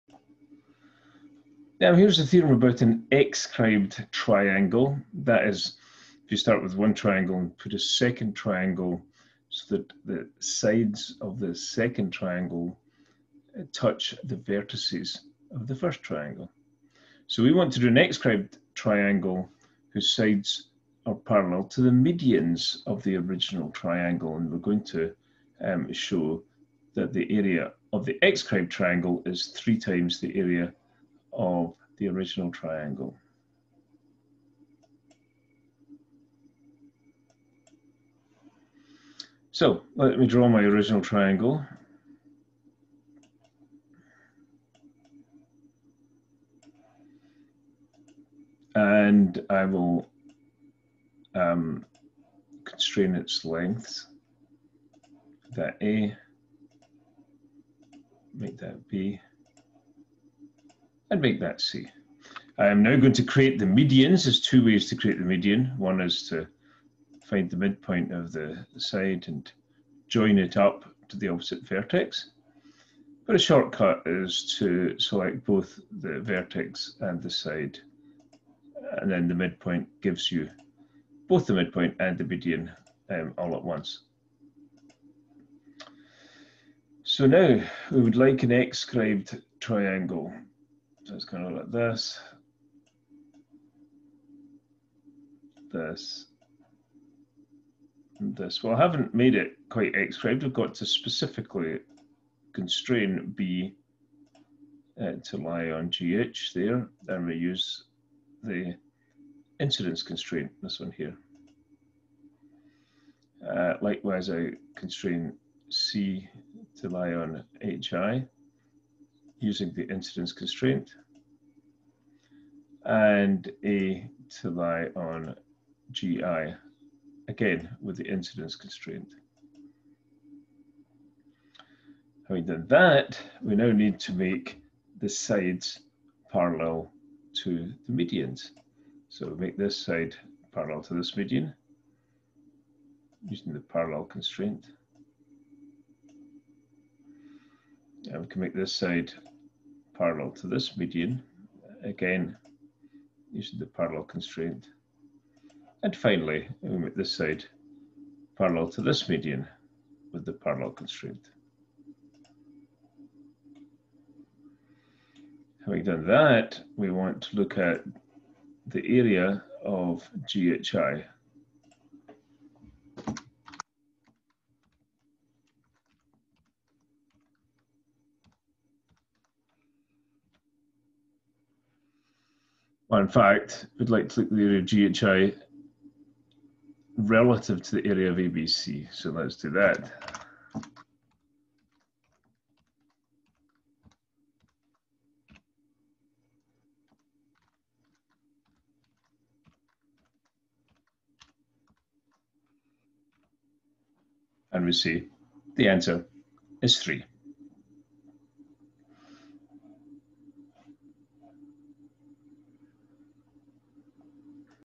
Exscribed Triangle
Show that the parallels through the vertices A, B, C of the triangle ABC to the medians of this triangle issued from the vertices B, C, A respectively form a triangle whose area is three times the area of the original triangle.